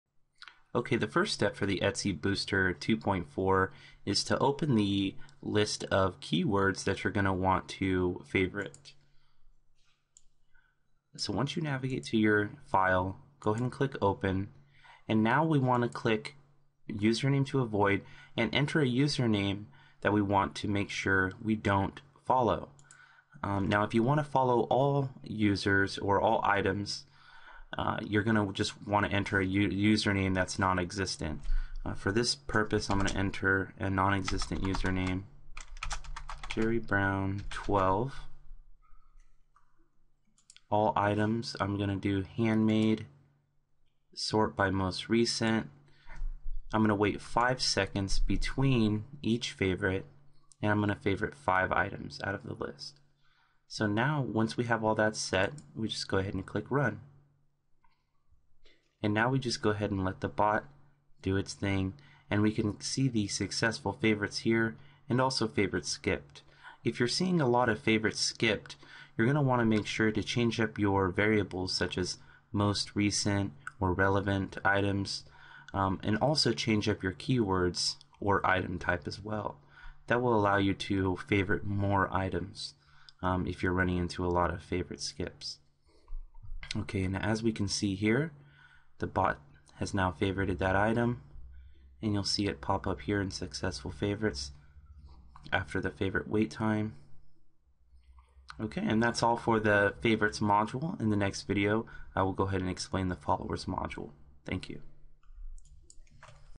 Etsy Booster v2.4 Favorites Instructions
- The first step is to populate the list of keywords which you will be using (one keyword or phrase per line). After which you can load the keyword list into the bot by clicking the Choose File button

- Enter the username you would like to avoid from adding to your favorites by entering it in the box labeled Username To Avoid. It is crucial that you enter a random user for example JerryBrown12 in order to favorite all items.

- Choose from the drop down menu Item Type you would like to favorite.

- Sort items by using the drop down to select Most Recent, Relevancy, Highest Price, Lowest Price.

- Enter a custom wait time for the bot to wait between the next favorite into the box labeled Favorite Wait Time.

- Choose the number of items you want to favorite by using the drop down menu labeled Favorites.

- Click Run and wait for Favorites Done! Alert.

Note: You can monitor the amount of successful favorites added under Successful Favorites. If you are noticing a lot of Favorites Skipped then it's a good idea to change your variables, meaning; keywords, item type, and sort by. This will help to increase the number of items you will be able to favorite using the bot.

Note: In order for the Favorites & Messenger module to work correctly you must enter a user name into the box you don't want to follow, for example; JerryBrown12